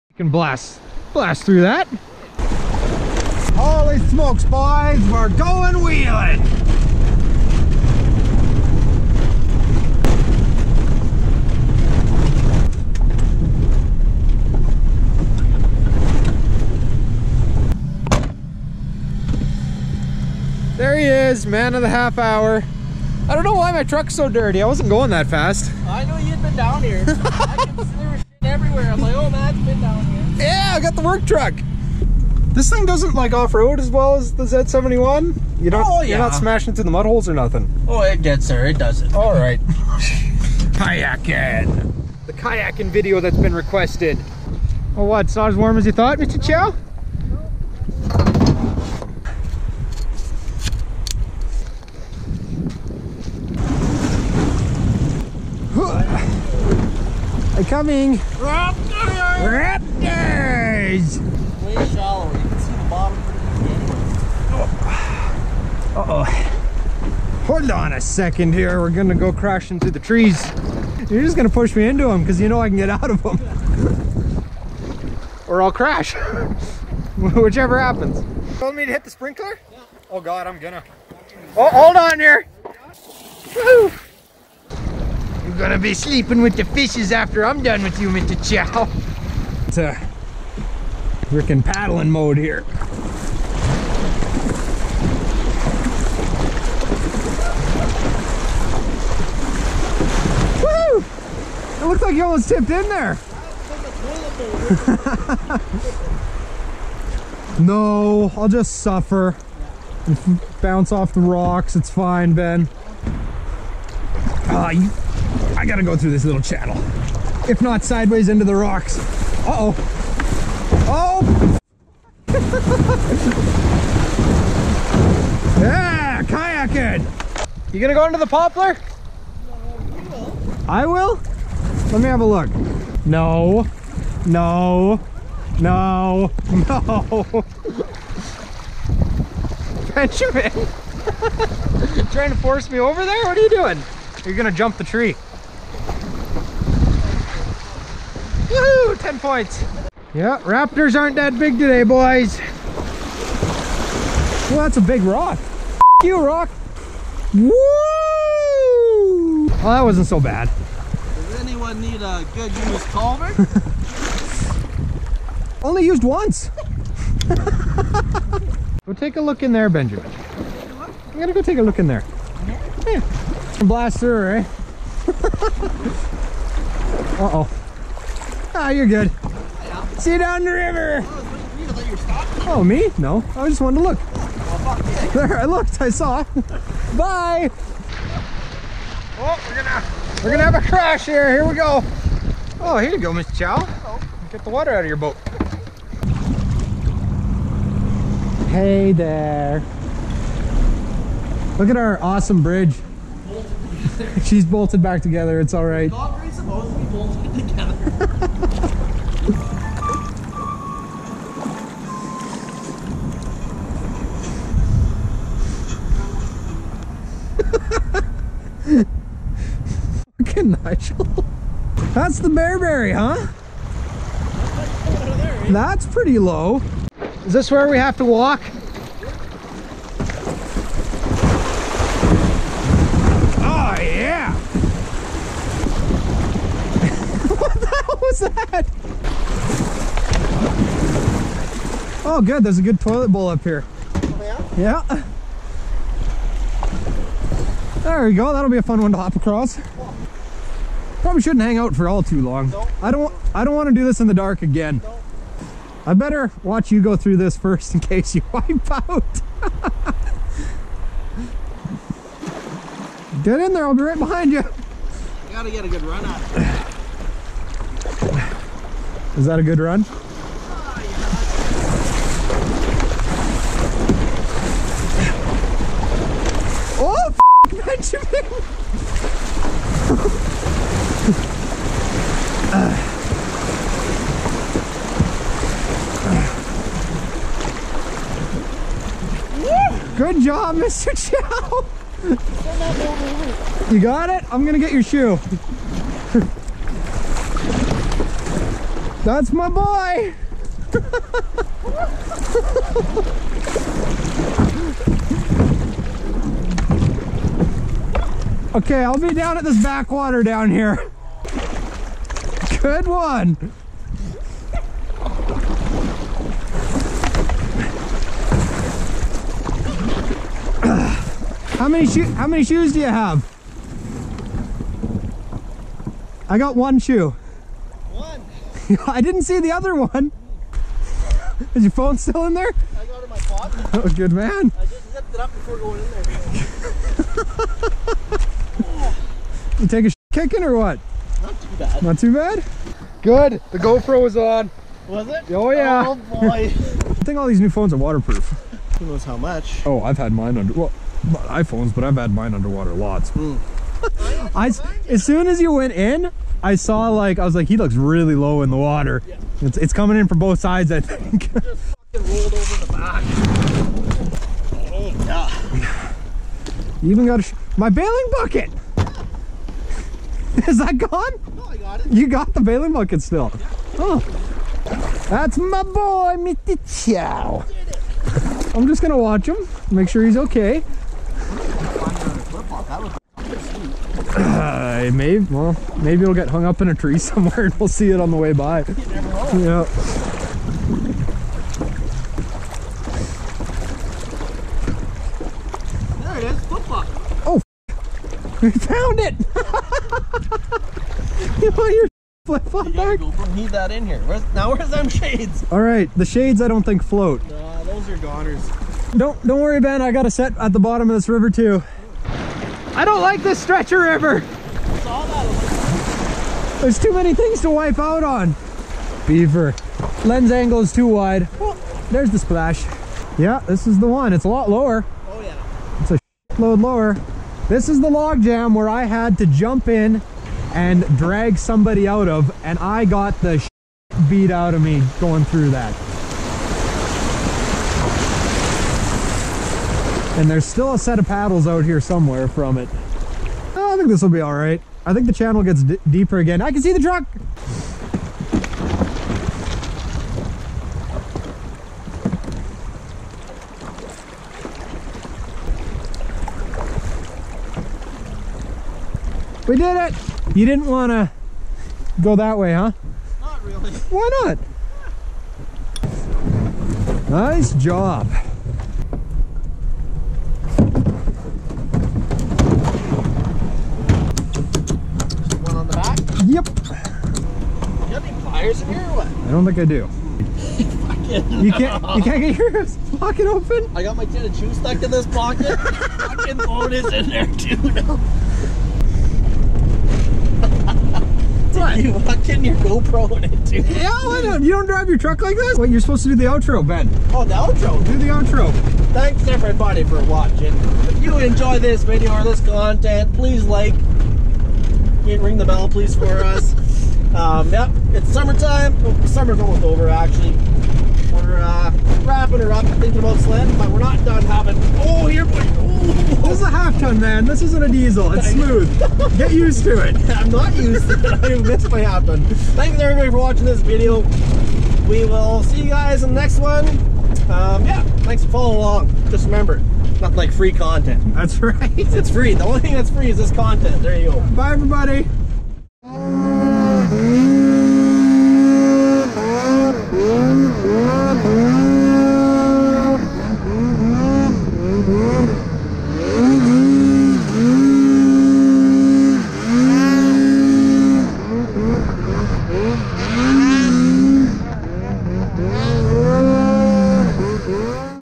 KAYAK FLIPS in the River
Going Kayaking down the Red Deer River! Ben ends up going into the drink after tipping in and taking a ride down the river without a boat!

Want to send something this way?
Get a shout-out maybe?
Send mail to:
Syxxes
Box 2108
Didsbury, Alberta T0M 0W0
Canada